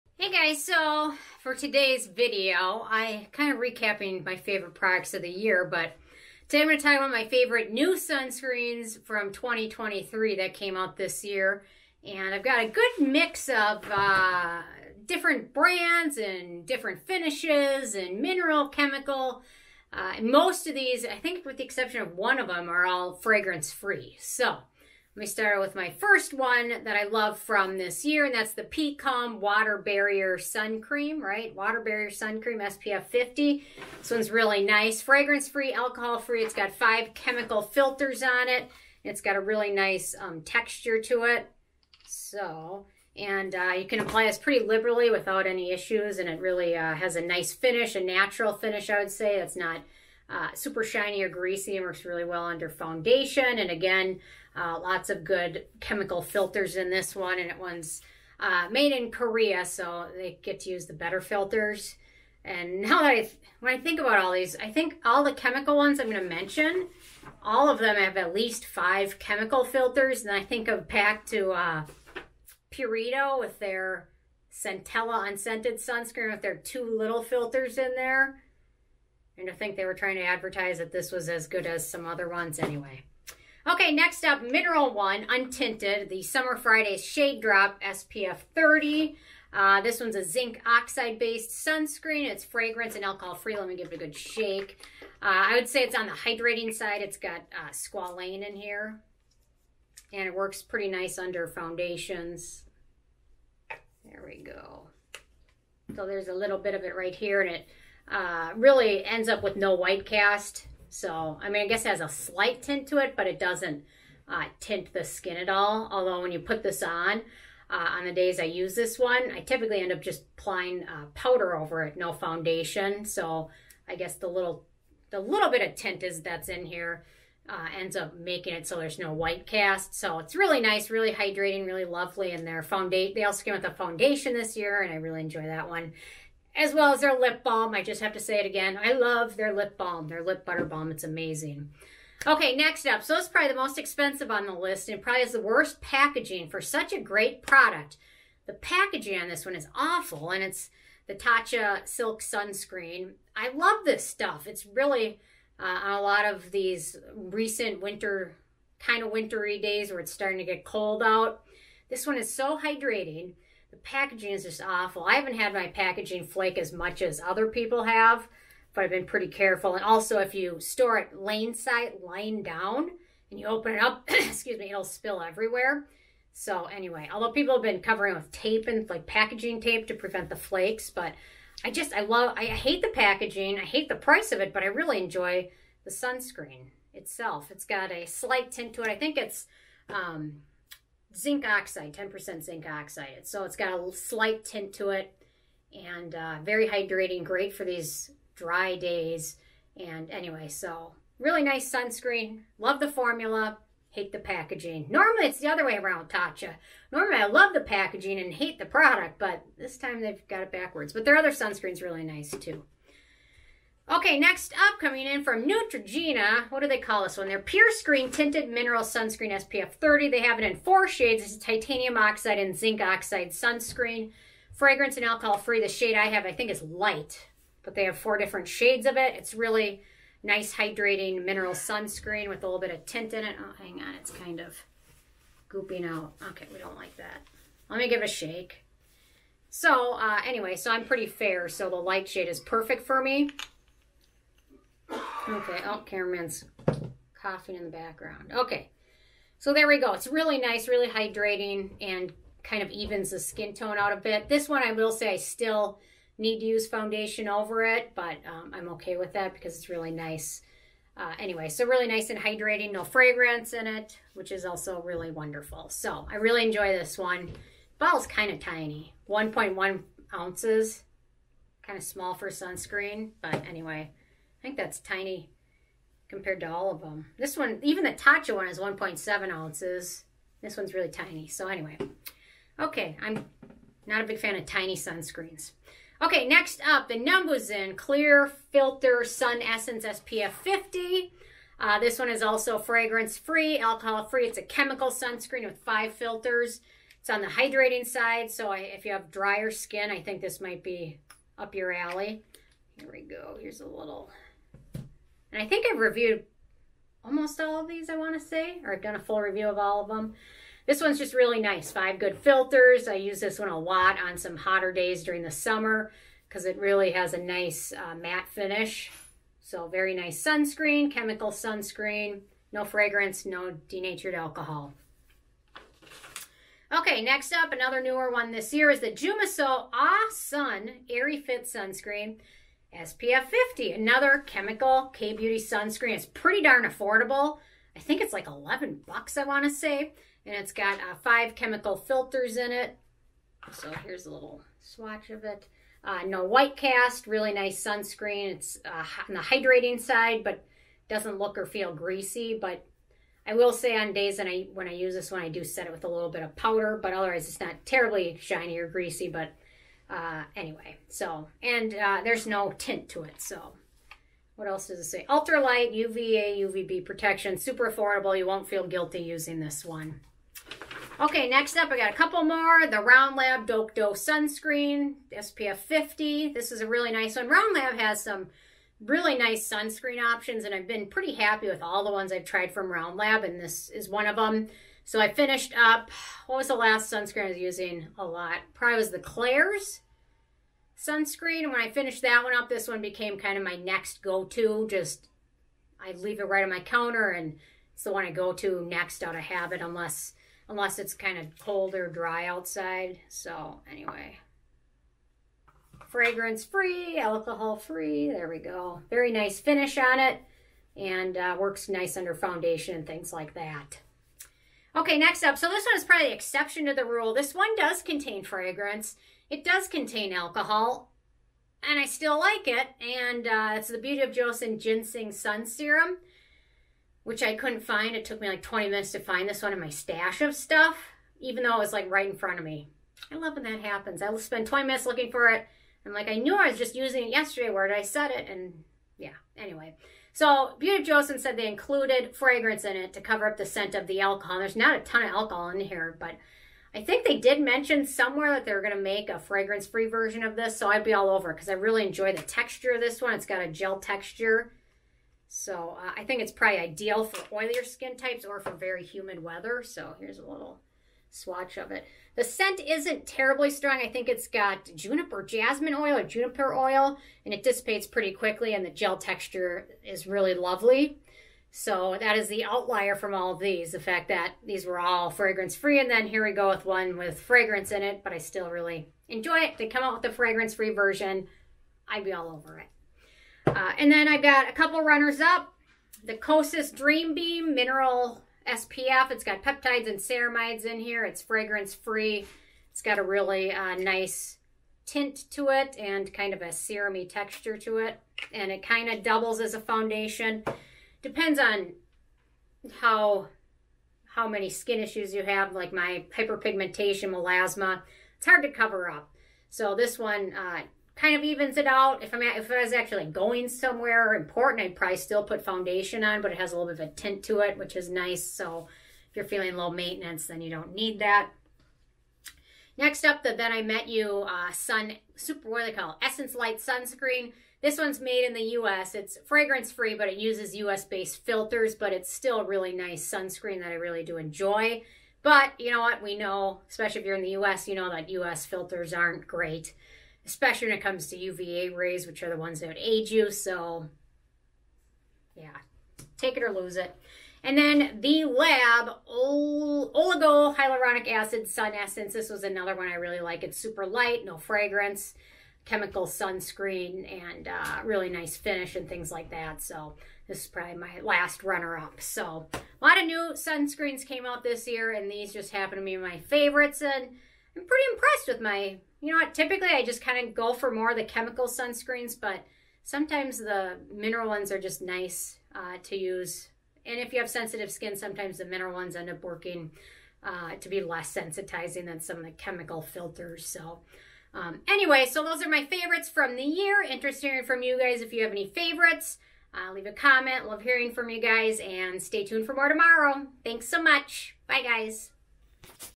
My Favorite New Sunscreens from 2023 🌞 Best Facial SPF's of the Year
- Best New Sunscreens of 2023

P Calm Water Barrier Sun Cream SPF 50
Fragrance & Alcohol Free
5 Chemical Filters
Use Rewards Code NOBSBEAUTY for an additional discount!

Summer Fridays ShadeDrops Mineral Milk Sunscreen SPF 30
Zinc Oxide based sunscreen
Fragrance & Alcohol Free
At Sephora:  see tagged products

Tatcha The Silk Sunscreen SPF 50
Terrible packaging, however, the actual product is wonderful.
Hydrating, fragrance free mineral sunscreen.

Neutrogena Purescreen+ Tinted Mineral Sunscreen SPF 30
Titanium Dioxide (3.2), Zinc Oxide (21.6) Mineral Sunscreen
Fragrance & alcohol free
At Ulta:  see tagged products

numbuzin - No.1 Clear Filter Sun Essence SPF 50
Fragrance & Alcohol free
Chemical Sunscreen with 5 filters

JUMISO - Awe-Sun Airy-Fit Sunscreen SPF 50
Fragrance & Alcohol Free
Chemical SPF with 5 filters

ROUND LAB - 1025 Dokdo Sunscreen SPF 50
Fragrance & alcohol free

Beauty of Joseon - Ginseng Moist Sun Serum
Does contain fragrance and a denatured alcohol
Clear gel texture, chemical SPF with 5 filters